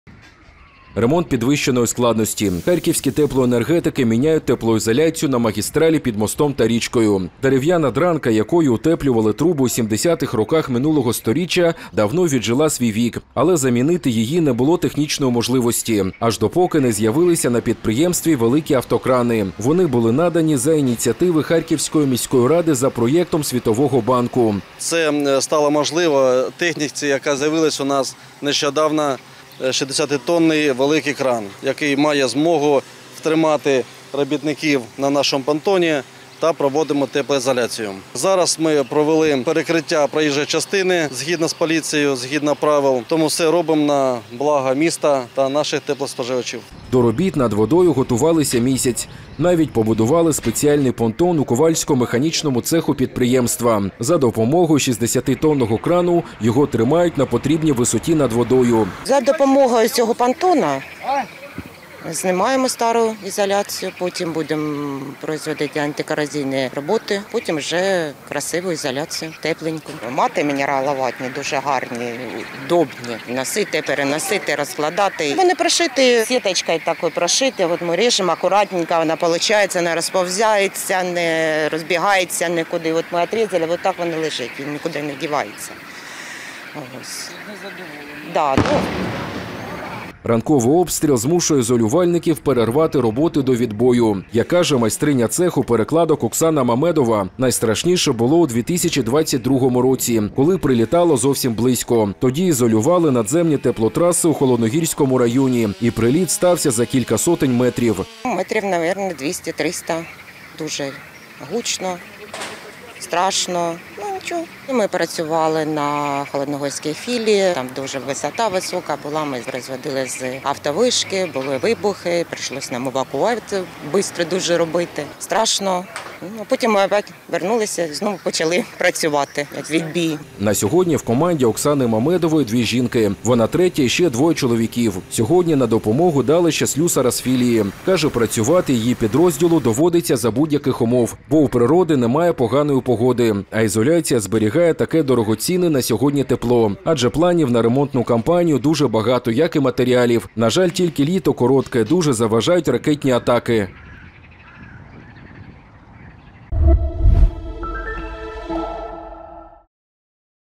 Харківські теплоенергетики міняють теплоізоляцію на магістралі під мостом над річкою
Ремонт підвищеної складності: харківські теплоенергетики міняють  теплоізоляцію на магістралі під мостом над річкою.  Дерев’яна дранка, якою утеплювали труби у 70-х роках минулого сторіччя давно віджила свій вік, але замінити її не було технічної  можливості, аж допоки не з’явилися  на підприємстві великі автокрани. Вони були надані за ініціативи  Харківської міської ради за проєктом Світового банку.